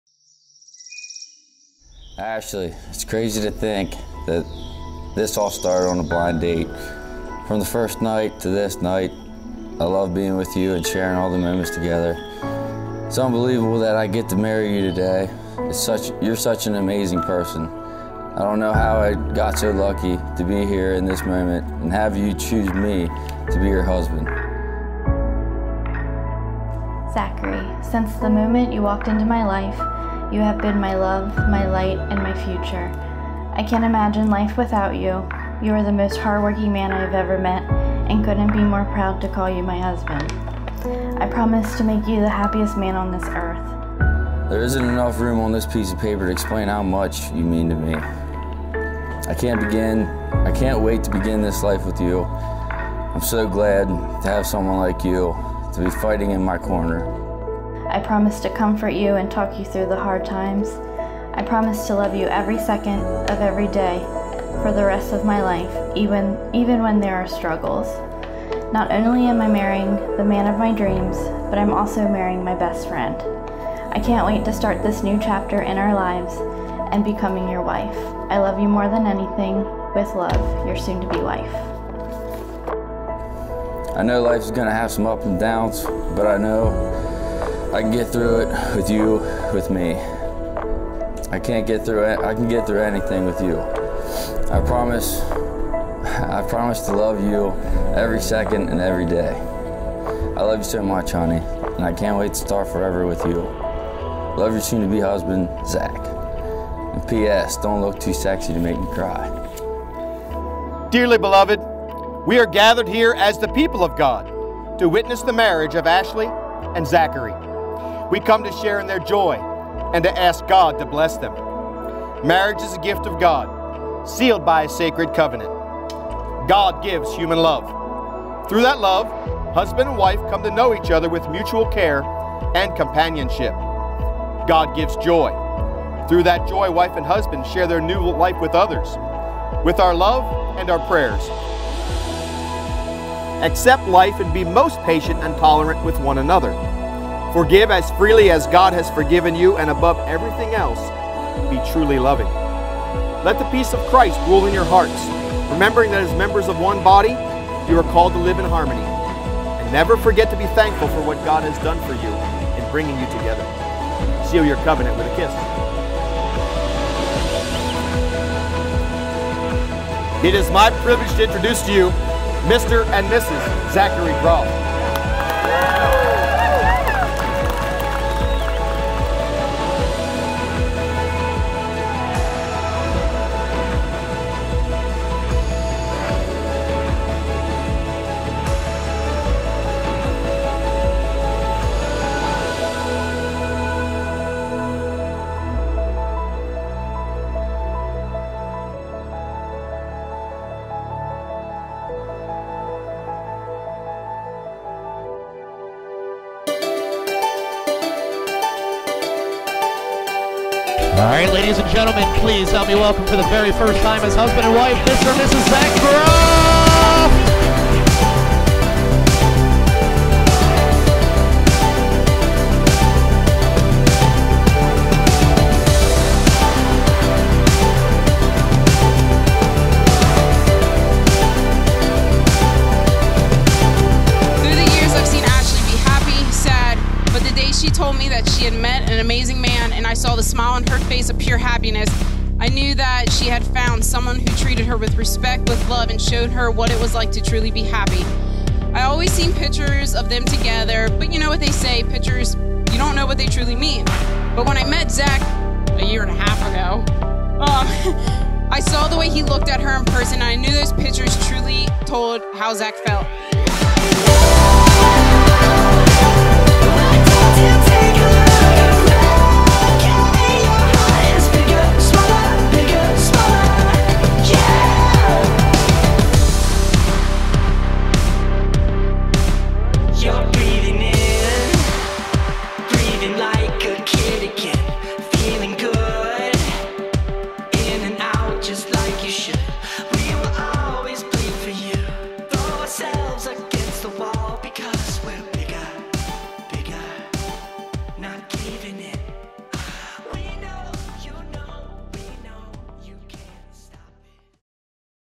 Ash+Zack | Two Harts Barn | Cinematic Wedding Film
Wedding Film | Ashley & Zack

"You don't marry the person you can live with—you marry the person you can't live without." -Unknown

Congratulations to this awesome couple!

Wedding Professionals
Venue | Two Harts Barn
Photographer | Herring Photography
Make Up | Collado Cosmetic Lab
Hair | Chelsi Groft
DJ | Rian Gomulka
Caterer | Skip and Penny's Country Catering
Cake | Jeannette Noel
Flowers | Bride's Mom, Jeannie Palmer Baublitz

[ Please watch in HD & SHARE ]